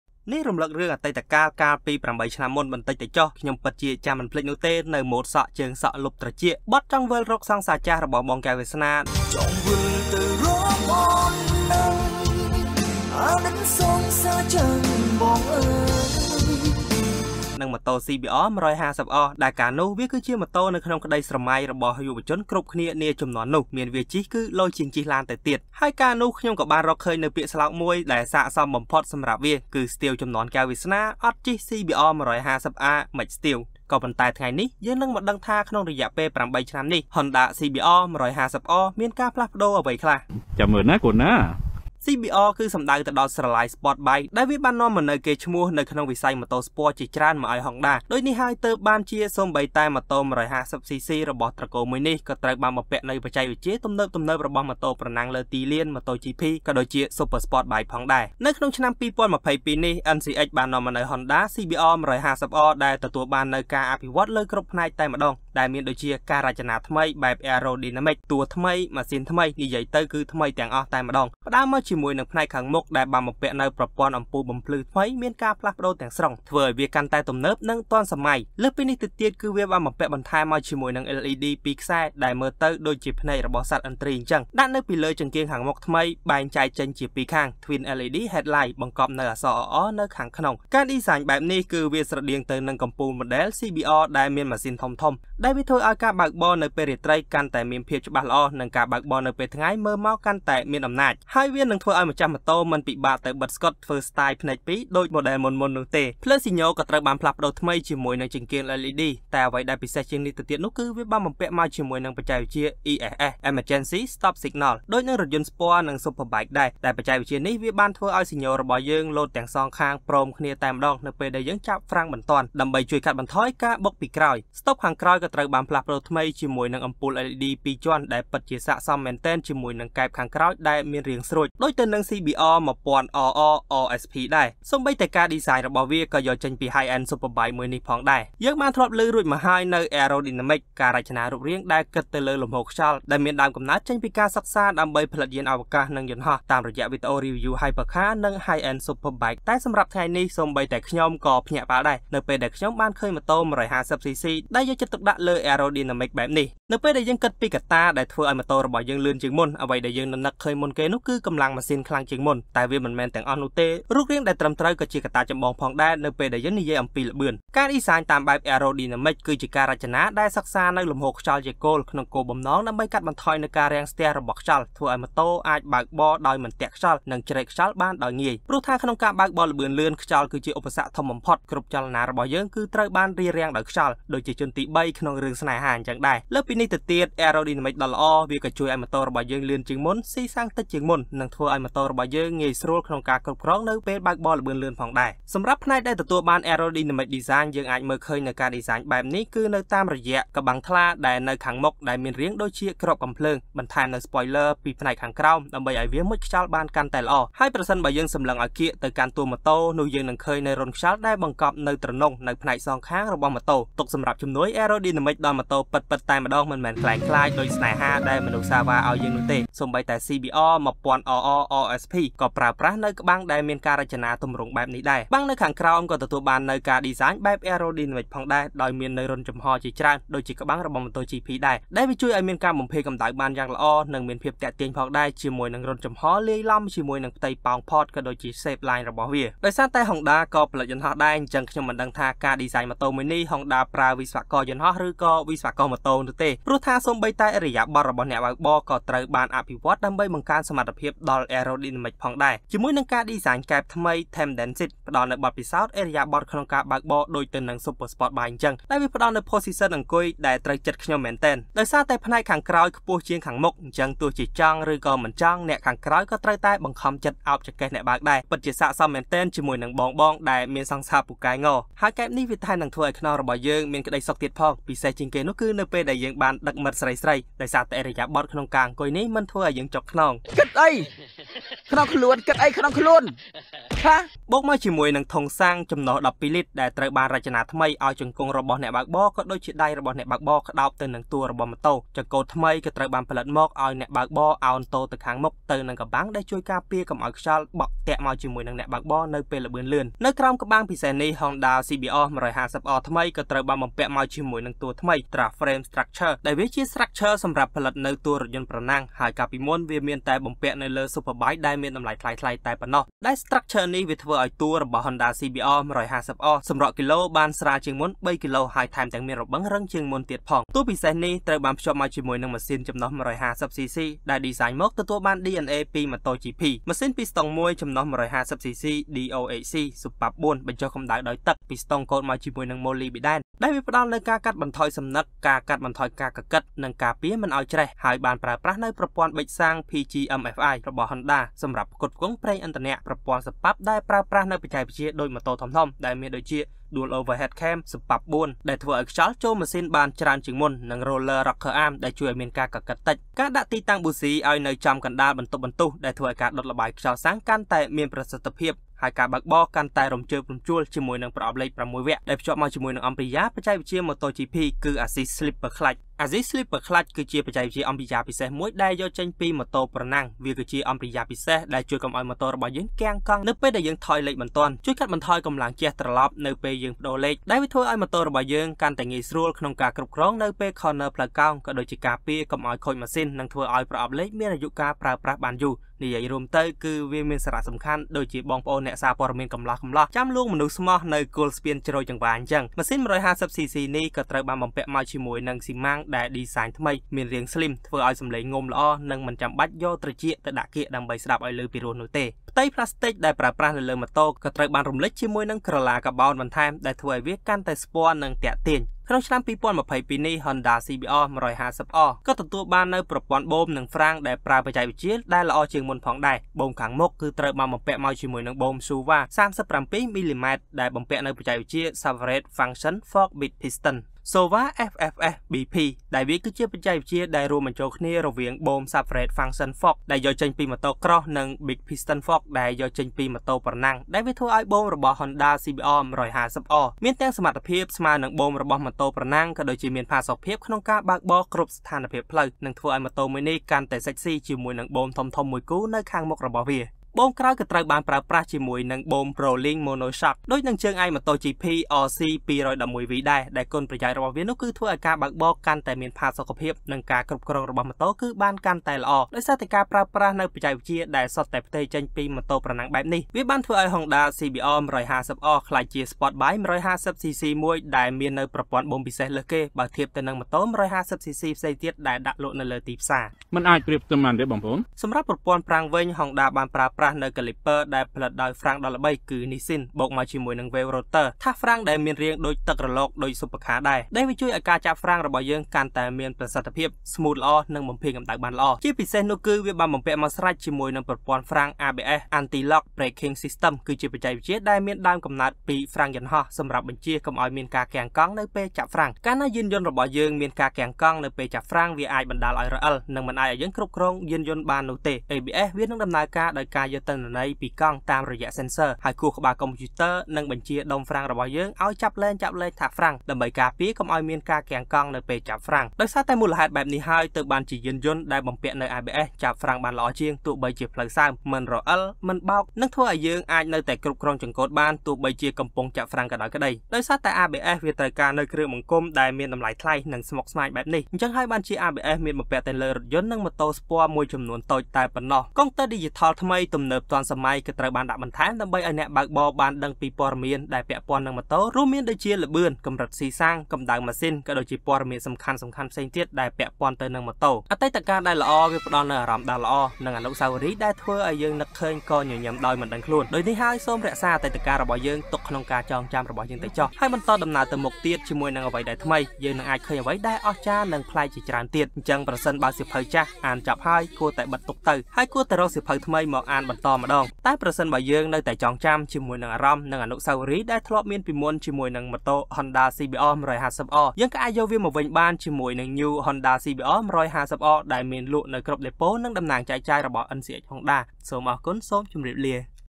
Honda CBR150R ការត្រលប់មកវិញដ៏អស្ចារ្យ : The Legend Reborn
យើងបានធ្វើការ Review  រួចមកហើយនៅប្រភេទម៉ូតូ Honda MSX Grom ជាប្រភេទម៉ូតូ mini sport bike និង Honda CB150R  ជាប្រភេទម៉ូតូកាហ្វេបែបវិទ្យាសាស្រ្ត។ដែលផលិតផលរបស់ NCX Honda ទាំង២នេះវាបានបំពាក់មកជាមួយនិងបច្ចេកវិទ្យាទំនើបៗជាច្រើនមែនទែន។ក៏ប៉ុន្តែថ្ងៃនេះយើងនិងដឹងថាតើរយះ 8ឆ្នាំមកនេះ Honda CBR150R មានការផ្លាស់ផ្ដូរអ្វីខ្លះ។
សូមអរគុណទៅដល់  @HondaCambodiaNCX  ដែលបានផ្ដល់នៅពត៍មានសម្រាប់ផលិត Video មួយនេះ។